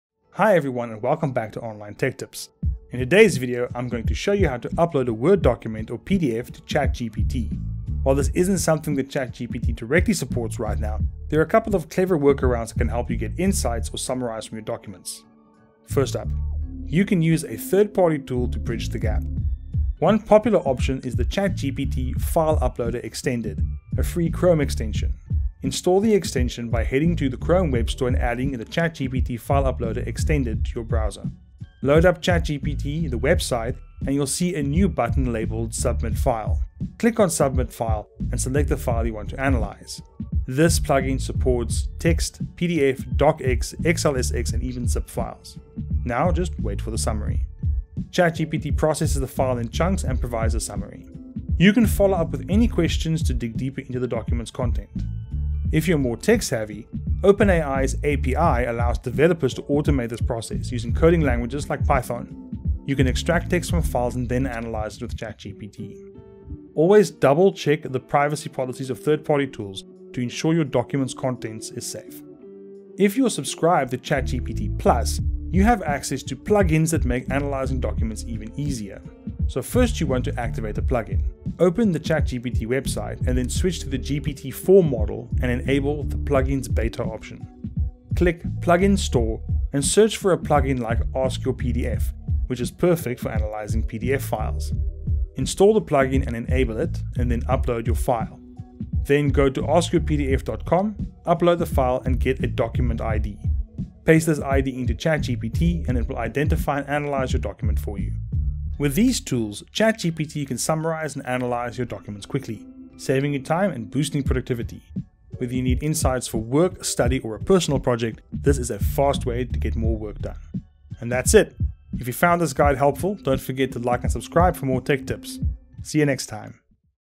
How To Upload Word Docs & PDFs to ChatGPT
ChatGPT is more than just a chatbot, you can do real work if you provide it with a PDF. It’s easier than it sounds, and we’ll show you how to get it working in minutes.

[Video produced for AKIC by @MiddWitt]